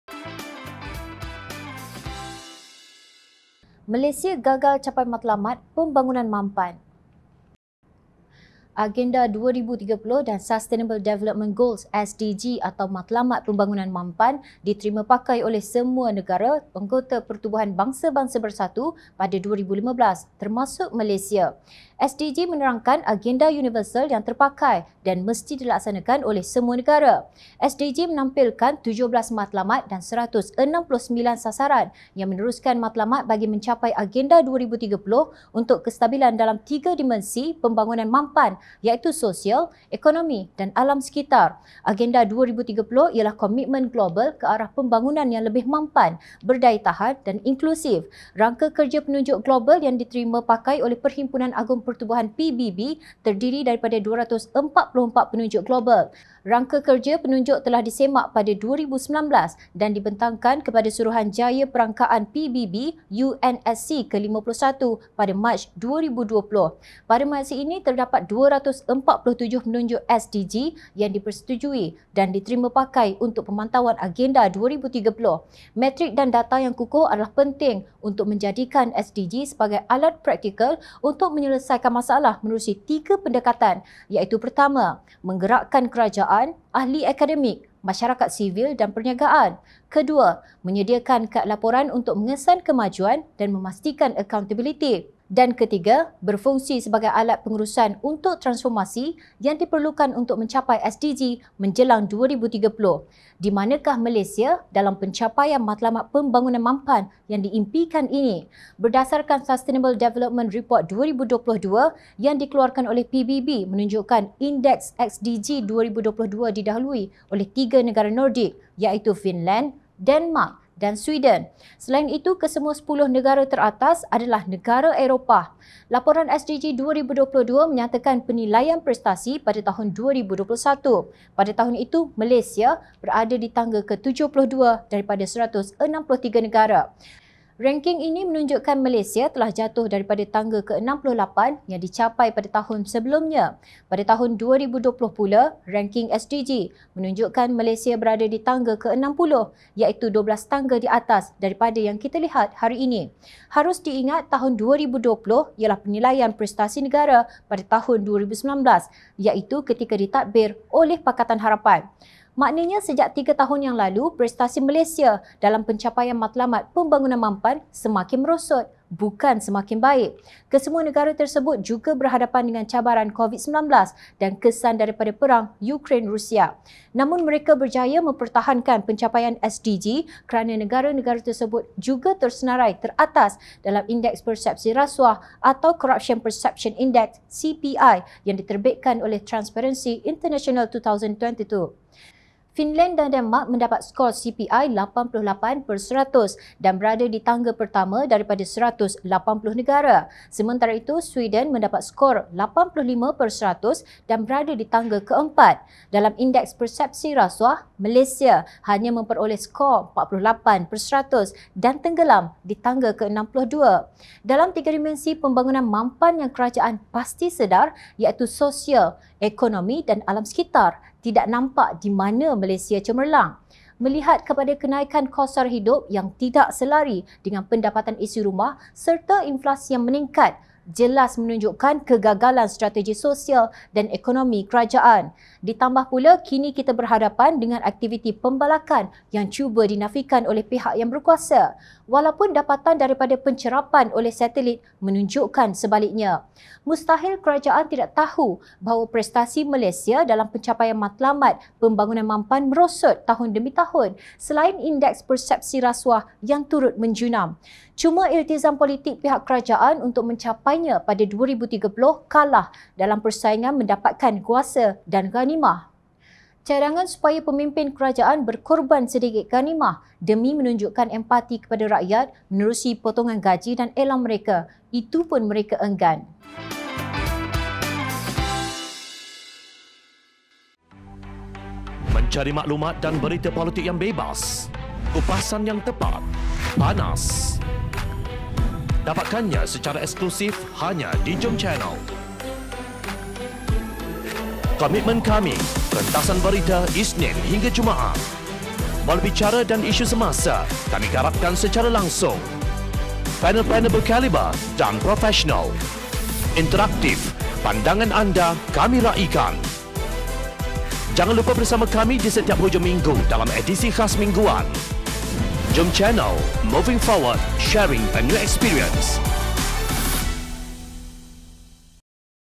MALAYSIA GAGAL CAPAI MATLAMAT PEMBANGUNAN MAMPAN
Rangka kerja penunjuk telah disemak pada 2019 dan dibentangkan kepada Suruhanjaya Perangkaan PBB (UNSC) ke-51 pada Mac 2020.

Pada masa ini, terdapat 247 penunjuk SDG yang dipersetujui dan diterima pakai untuk pemantauan Agenda 2030.